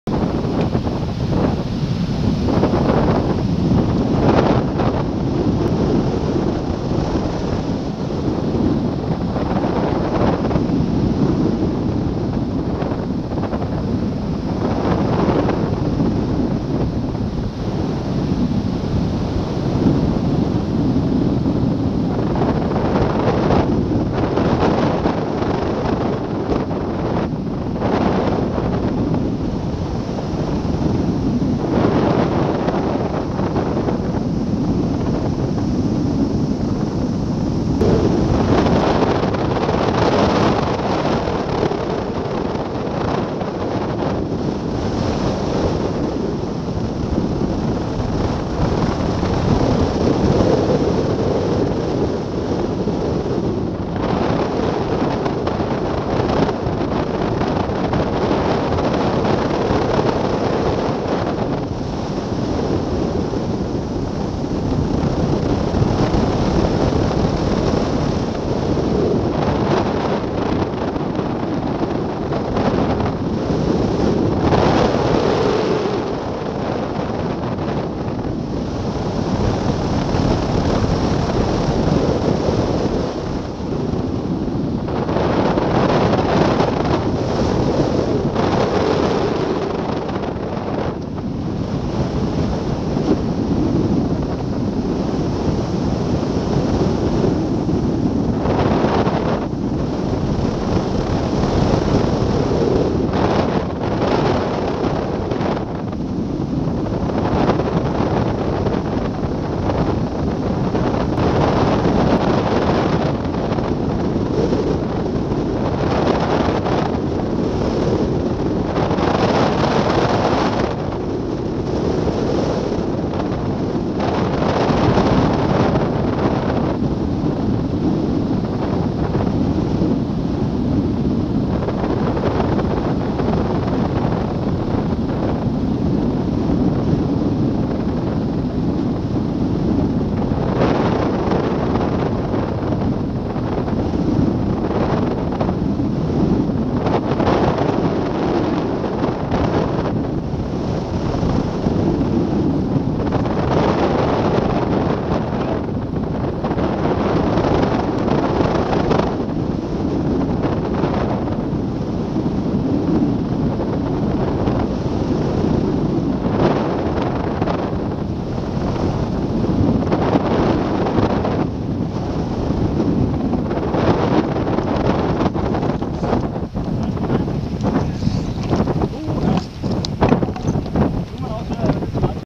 Hang gliding. Short flight to Vejby Strand and back.
A short coastal flight to Vejby Strand and back. Takeoff and toplanding at Rågeleje. Wind: NW, 8-10 m/s.